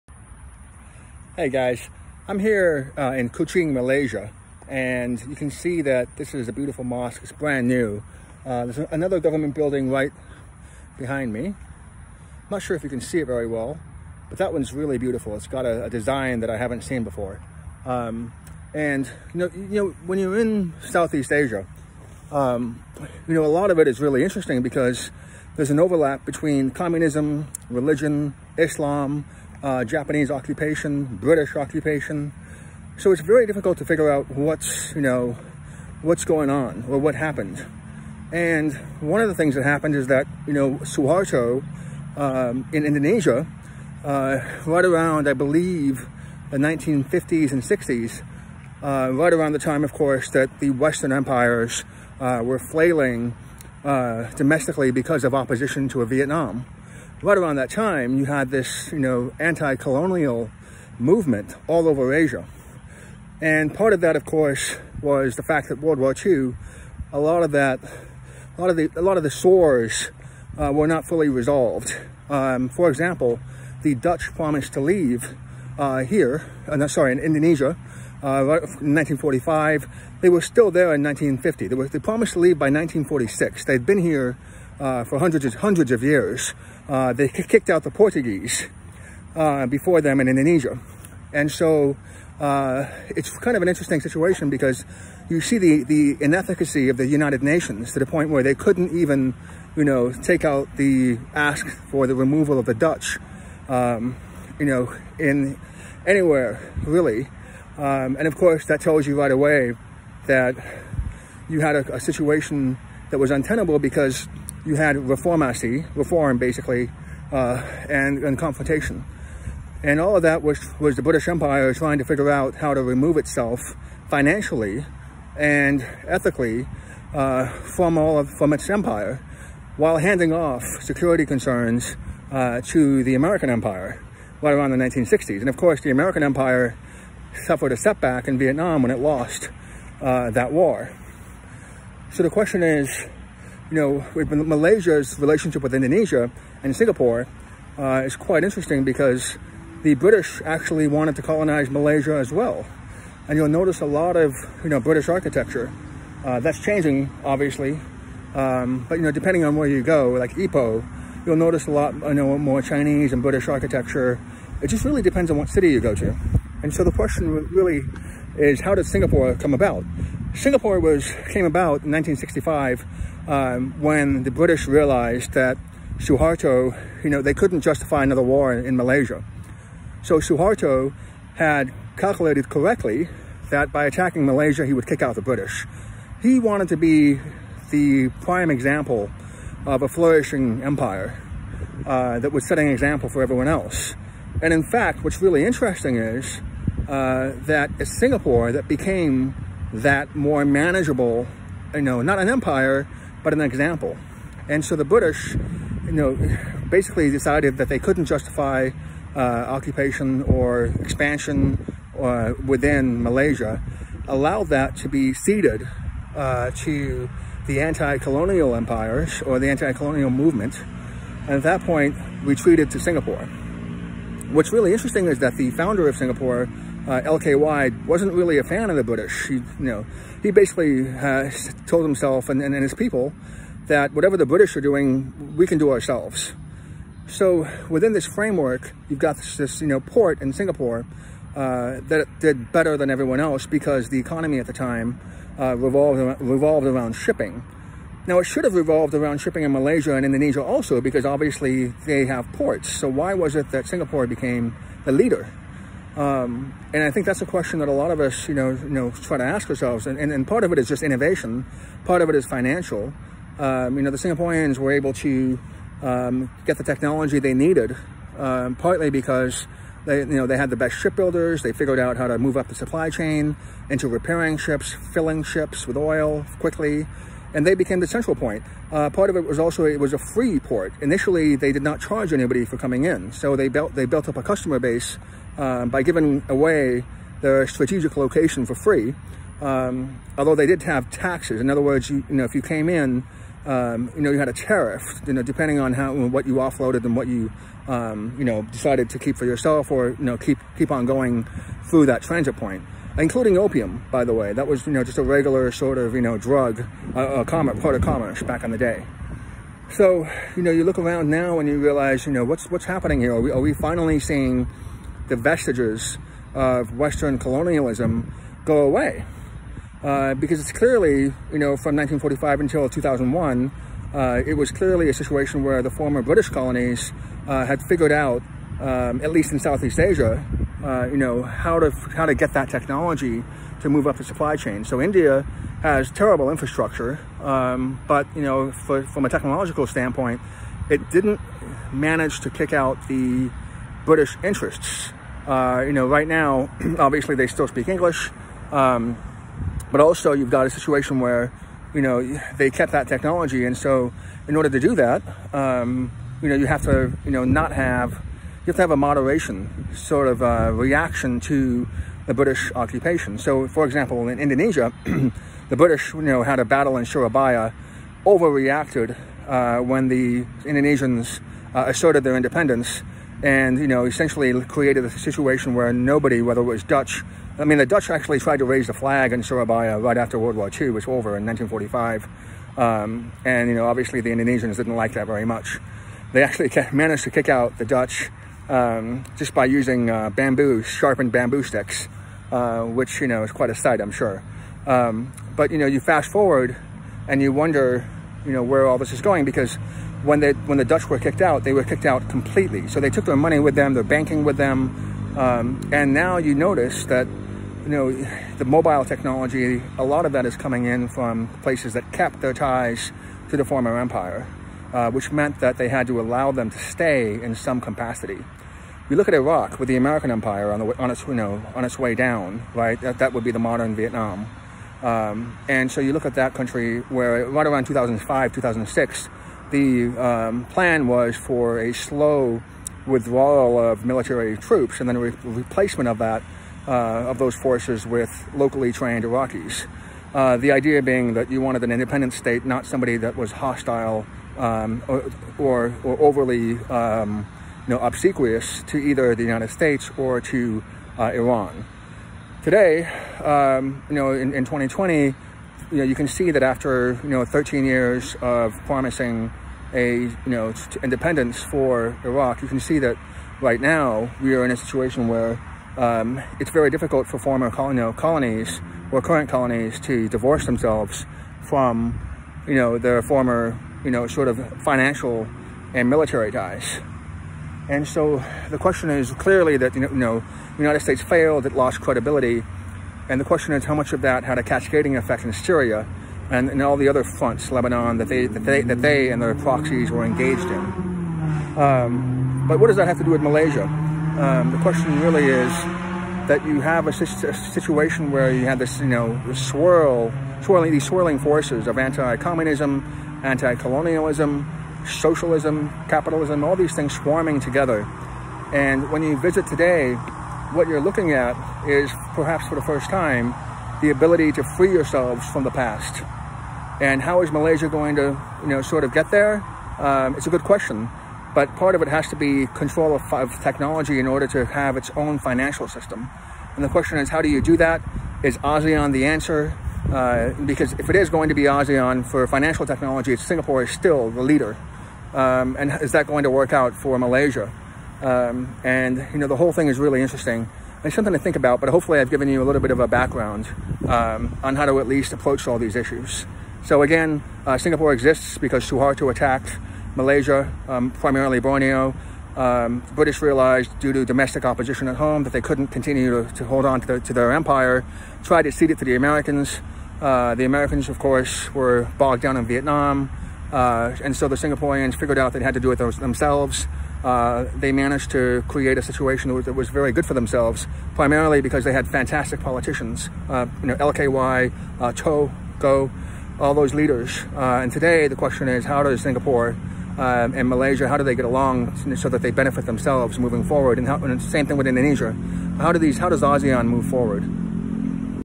Colonialism in SE Asia
The anti--colonial movement in the 50s and 60s and its impact are still with us today in 2020. (January 2020)

Correction 1: I meant Konfrontasi and Reformasi.

Correction 2: about 40,000 to 50,000 Dutch were expelled from Indonesia, though many others left on their own.

Correction 3: To be clear, Sukarno attacked Borneo, and Suharto settled the dispute without escalation when he came into power, replacing Sukarno. As part of the replacement, it appears Suharto was promised international development funds.